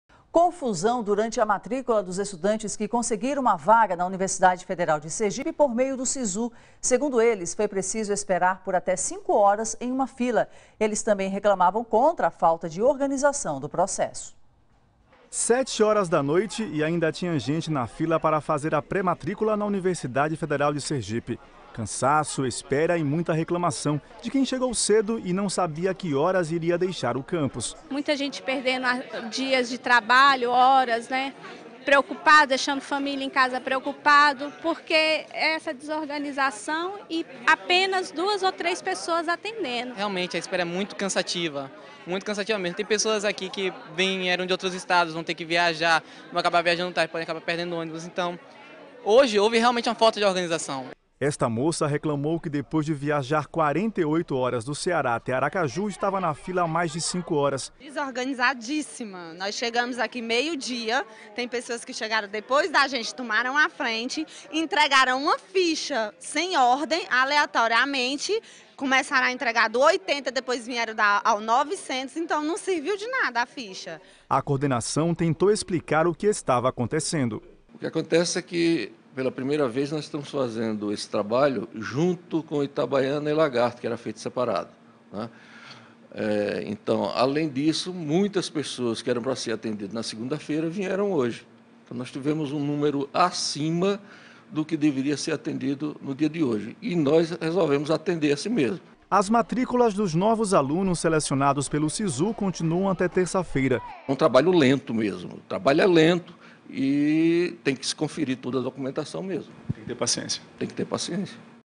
Acervo | Estudantes que conseguiram vaga na UFS pelo Sisu esperam mais de 5 horas para matrícula
Data: 18 jan 2014.